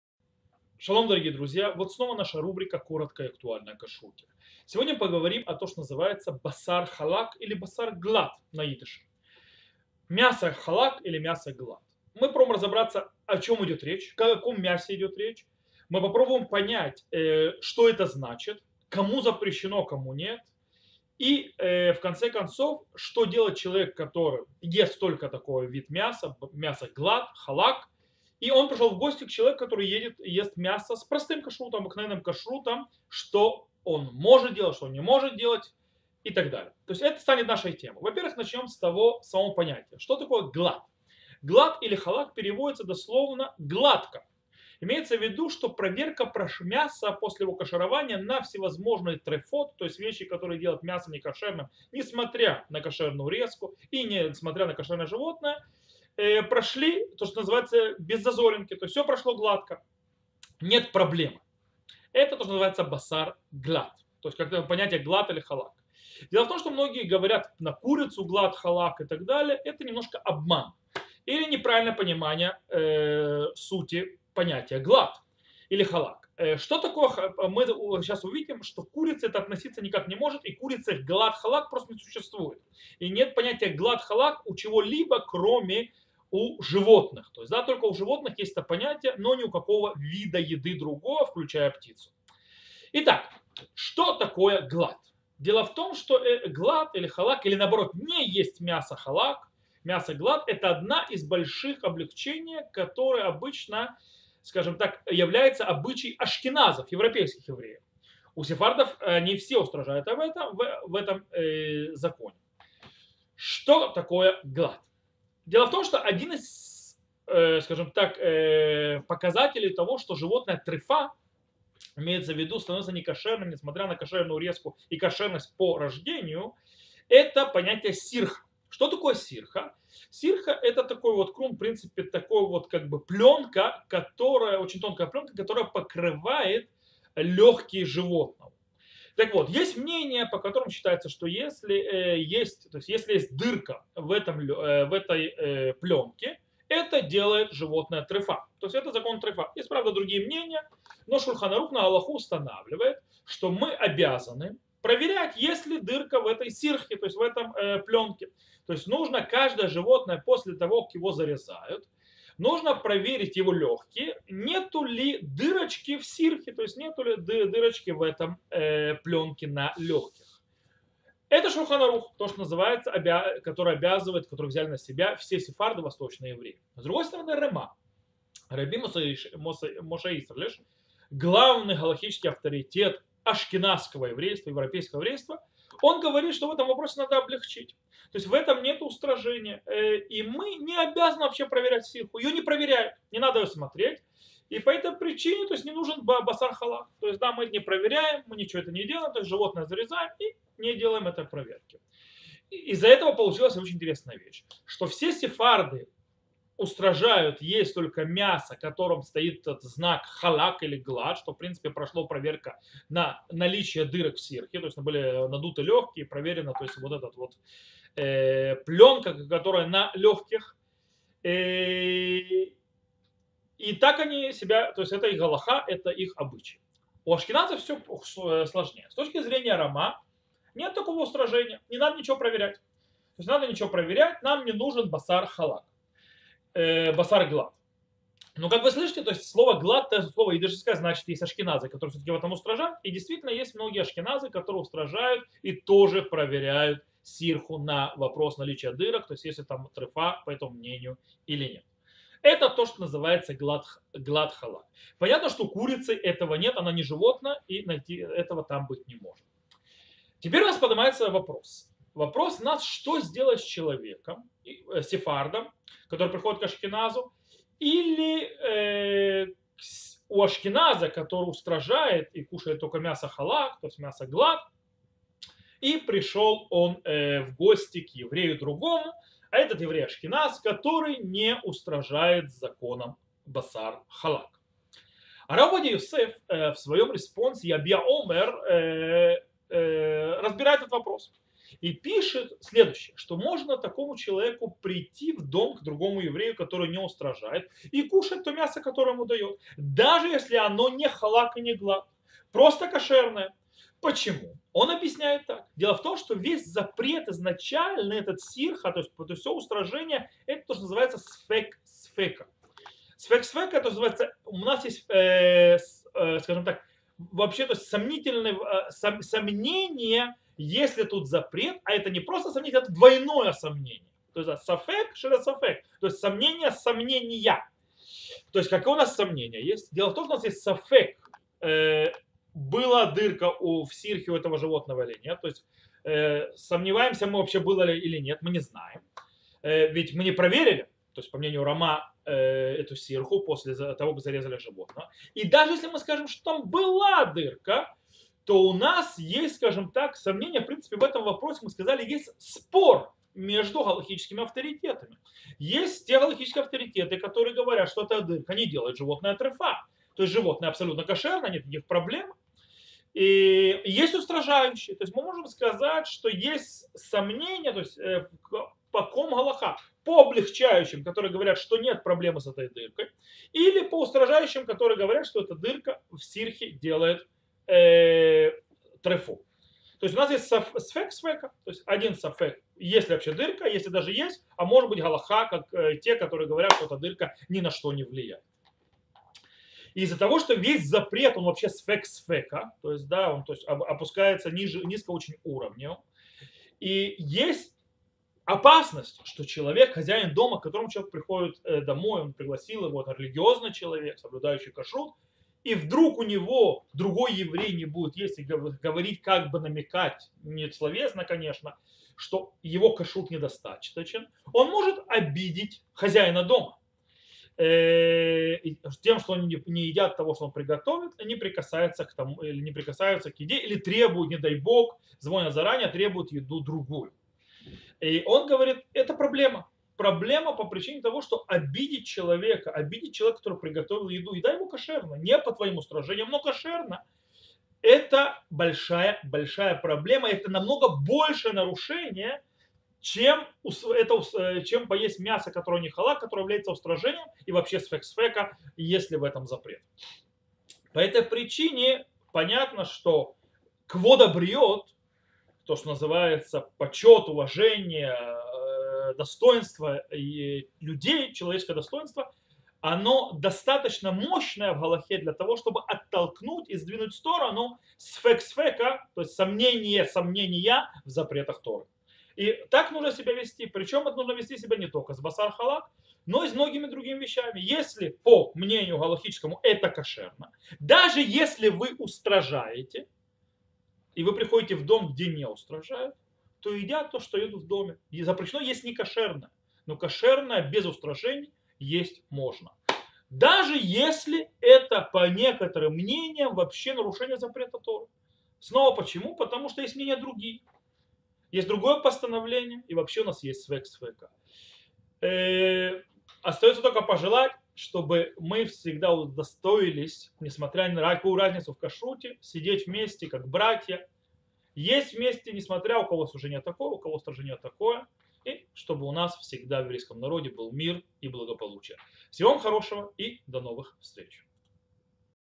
🍽️ Мясо глат халак: определение и законы | Коротко и актуально о кашруте. Урок 30
Шалом, друзья! Представляем вам новый урок рава Бриска из серии, обсуждающей акуальные вопросы кашрута в кратком формате!

- Что называется глат или халак? Что такое мясо глат-халак?
- Кто обязан есть только такое мясо, а кто в принципе нет?
- Что делать человеку, устрожающему в законах мяса глат-халак, пришедшему в гости к еврею, который идет за облегчающим мнением в этом вопросе? И на базе этого как себя вести гостю, устрожающему в законах кашрута, когда он в гостях у того, кто идет по облегченным мнениям?

Надеемся, этот материал был вам интересен и полезен! Задавайте ваши вопросы!